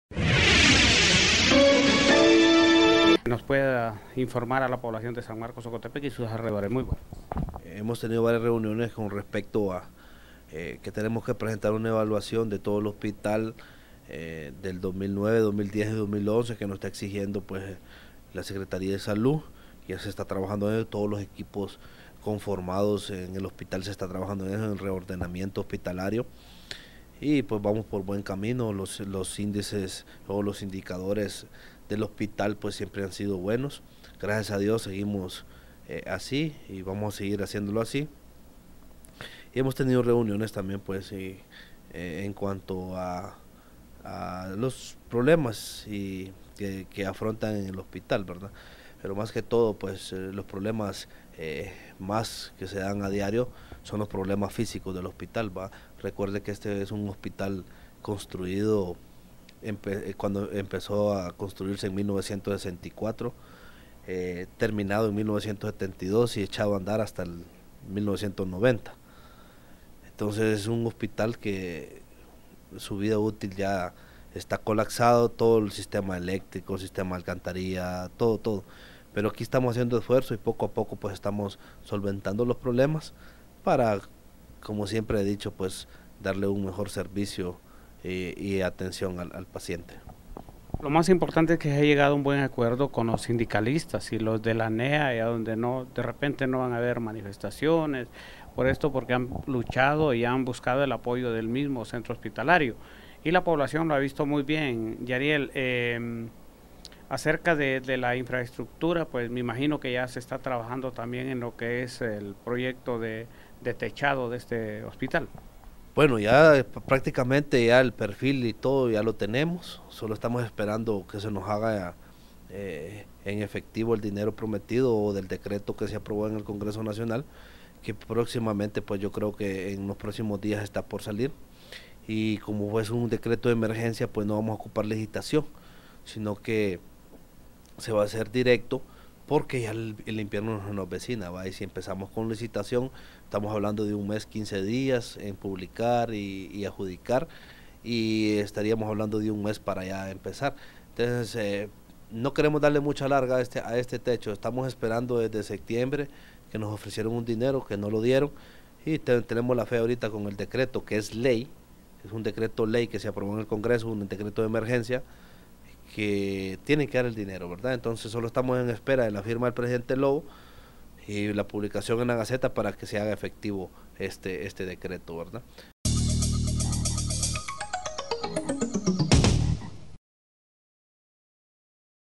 Avance Noticioso sanmarcostv _06 Marzo_Edición 2
Lic. Yariel López, Nos habla de cómo esperan lo que les ha prometido el congreso nacional para las mejorías en el Hospital...